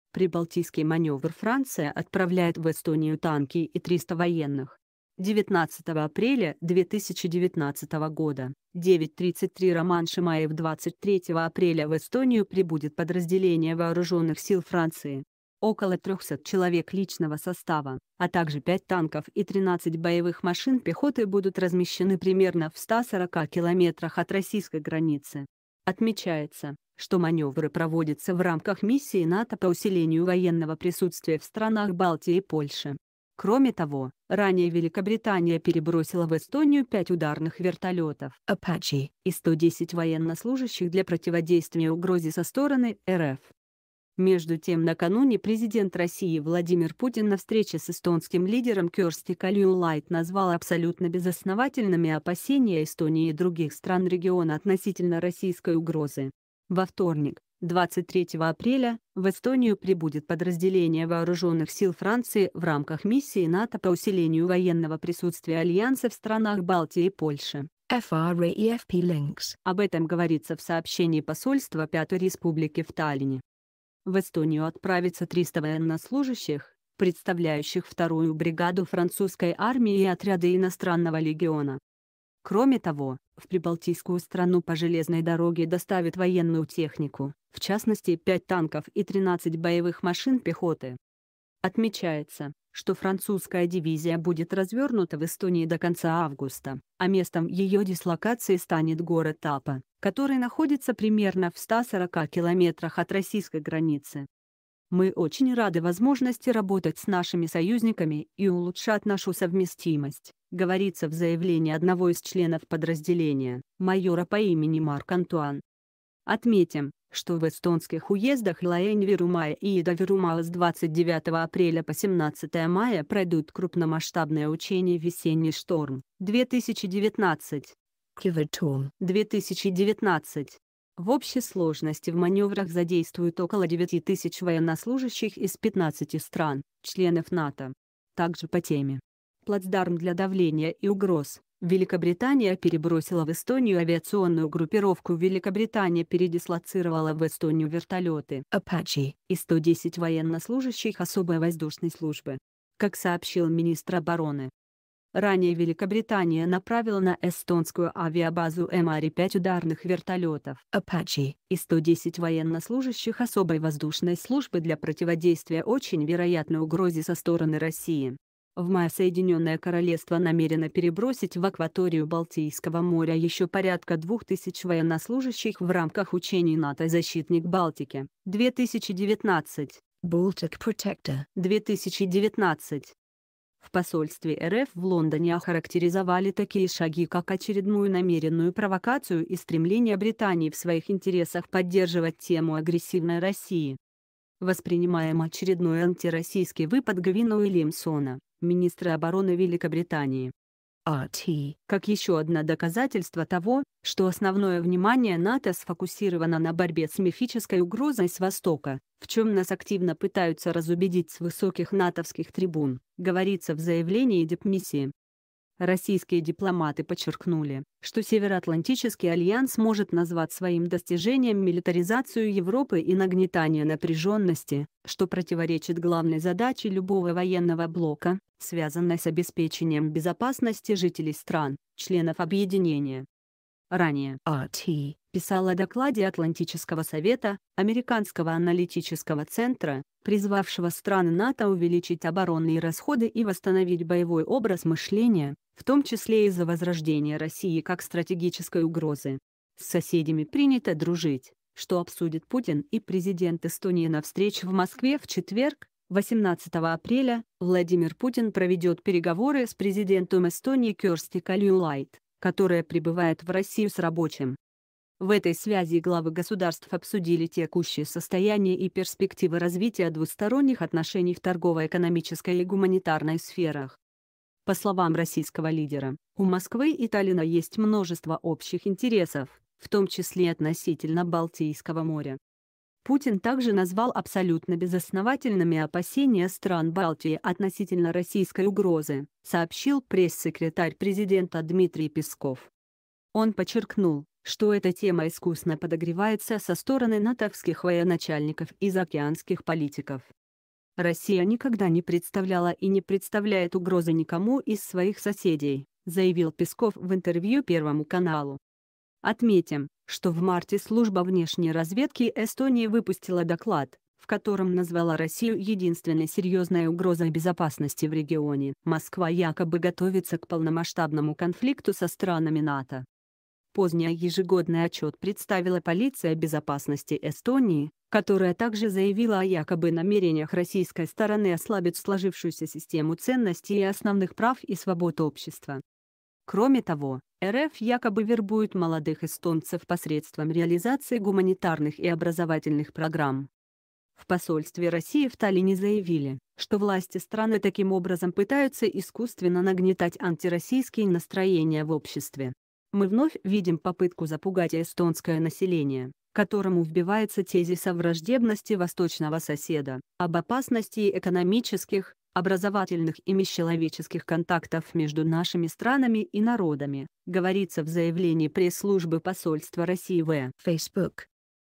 Прибалтийский манёвр Франция отправляет в Эстонию танки и 300 военных
Прибалтийский манёвр Франция отправляет в Эстонию танки и 300 военных 19 апреля 2019, 09 33 Роман Шимаев 23 апреля в Эстонию прибудет подразделение Вооружённых сил Франции . Около 300 человек личного состава, а также пять танков и 13 боевых машин пехоты будут размещены примерно в 140 км от российской границы . Отмечается, что манёвры проводятся в рамках миссии НАТО по усилению военного присутствия в странах Балтии и Польше . Кроме того, ранее Великобритания перебросила в Эстонию пять ударных вертолё